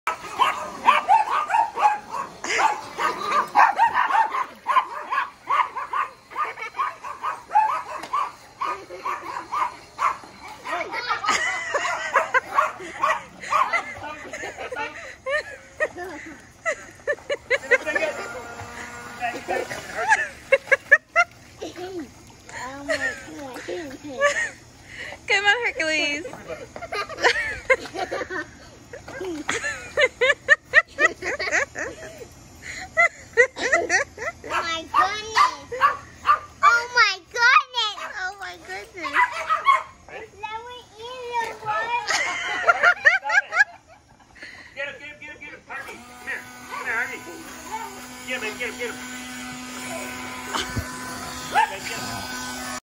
Tyco Riptide vs Yorkshire Terrier
First day with the Riptide and the doggies loved it!!